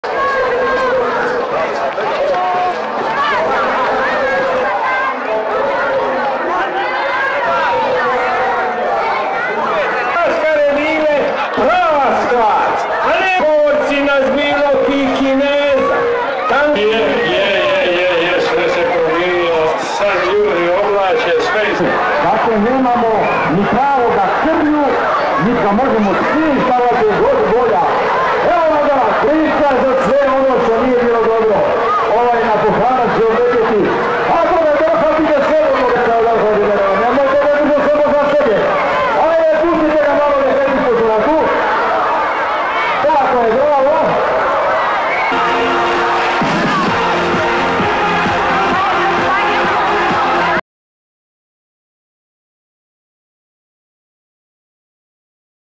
Maškarana povorka dječjih vrtića i škola na trgu Poljana
Šibenik, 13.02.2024 - Maškarana povorka dječjih vrtića i škola na trgu Poljana. Video: Nikša Stipaničev / CROPIX